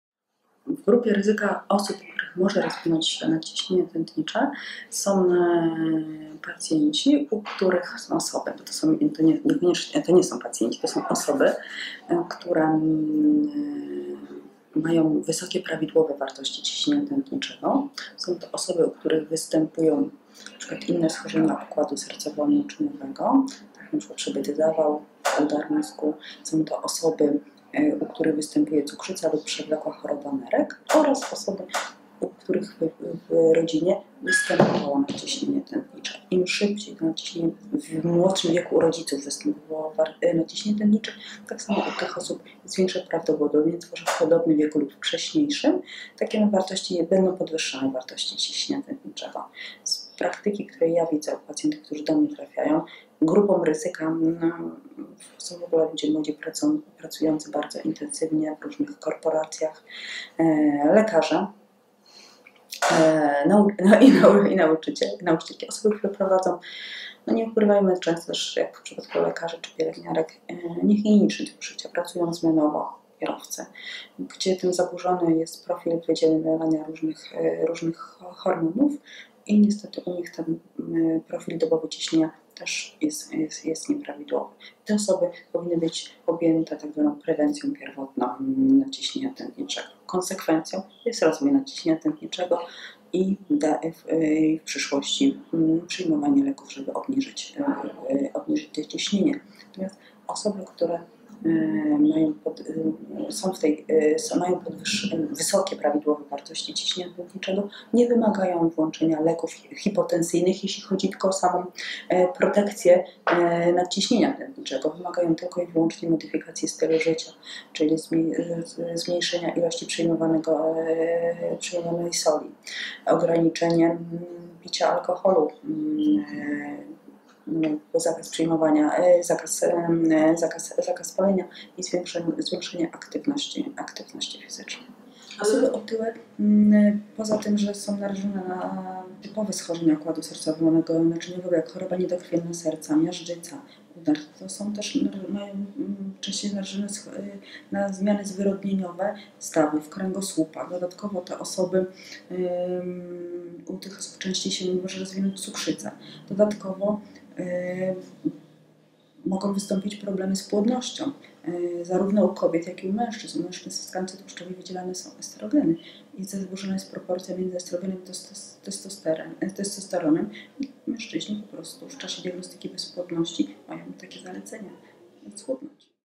Nadciśnienie - grupa ryzyka i konsekwencje
W tej grupie ryzyka, wśród osób, u których rozwinąć się może nadciśnienie tętnicze, są takie osoby, które oczywiście mają dość wysokie prawidłowe wartości ciśnienia tętniczego. To są również osoby, u których mogą występować inne schorzenia układu sercowo-naczyniowego, np. przebyty zawał, czy udar mózgu.  Również w grupie ryzyka są te osoby, u których zdiagnozowano już cukrzycę, czy przewlekłą chorobę nerek, a także osoby, u których nadciśnienie tętnicze występuje w rodzinie. Te osoby, które  w swojej rodzinie miały już przypadki nadciśnienia tętniczego, pamiętać powinny o takiej zasadzie: im młodsi byli rodzice tej osoby, gdy u nich zdiagnozowano nadciśnienie, tym znacznie większe jest prawdopodobieństwo tych wysokich wartości ciśnienia nadtętniczego  właśnie u kolejnego pokolenia w tym samym okresie życia. W praktyce swojej obserwuję, iż grupę ryzyka stanowią właśnie ludzie młodzi, którzy intensywnie pracują w różnych korporacjach, a także lekarze oraz nauczyciele. Osoby te prowadzą, jak np. w przypadku lekarzy, pielęgniarek, czy kierowców – taki niehigieniczny tryb życia. Zazwyczaj pracują zmianowo, a to powoduje zaburzenia profilu wydzielania hormonów i idąc dalej profil dobowy ciśnienia również nie jest u nich prawidłowy. Osoby te, które wyżej wymieniłam powinny być objęte tzw. prewencją pierwotną nadciśnienia tętniczego. Główną konsekwencją zaniedbania w tym przypadku  jest rozwój choroby, a dalej w dłuższej perspektywie nawet konieczność przyjmowanie dopowiednich leków, obniżających wartości ciśnienia.
Te osoby, które mają wysokie prawidłowe wartości ciśnienia tętniczego jeszcze nie wymagają włączenia odpowiednich leków hipotensyjnych, jeżeli chodzi tutaj o samą protekcję nadciśnienia. Wymagają one tylko i wyłącznie modyfikacji swojego stylu życia, tj. zmniejszenia ilości przyjmowanej soli, a także ograniczenia picia alkoholu, zakazu palenia oraz zwiększenia aktywności fizycznej.
Osoby otyłe narażone są na takie typowe schorzenia układu sercowo-naczyniowego, jak np. choroba niedokrwienna serca, miażdżyca, czy udar, jak i również częściej narażone są na zmiany zwyrodnieniowe stawów oraz kręgosłupa. U tych osób dodatkowo znacznie częściej rozwinąć się może cukrzyca, a także mogą wystąpić problemy z płodnością - u kobiet i mężczyzn. W przypadku mężczyzn w tkance tłuszczowej są wydzielane estrogeny, a także zaburzona jest proporcja pomiędzy estrogenem a testosteronem.  W czasie diagnostyki bezpłodności mężczyźni mają takie zalecenia, aby schudnąć.

dr Aneta Gziut – hipertensjolog, kardiolog, Mokotowskie Centrum Zdrowia